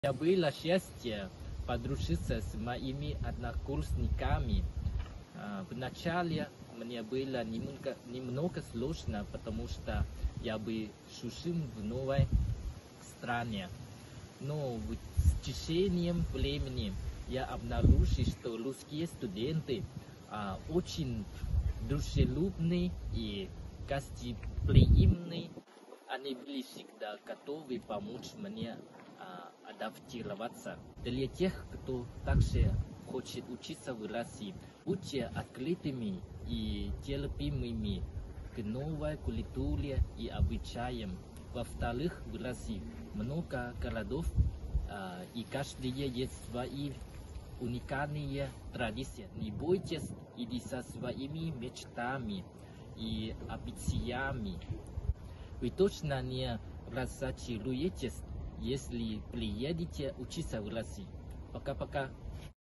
Обучение в России. Сайпанья Пхотхилат из Лаоса
💬 «Будьте открытыми и терпимыми к новой культуре и обычаям. Не бойтесь идти за своими мечтами», — с таким советом обратился Сайпанья Пхотхилат из Лаоса к тем, кто тоже хочет получить образование в России. Он учится в Томском государственном университете по специальности «Политология».

🎬 О студенческой жизни глазами иностранца - узнайте из нашего видео.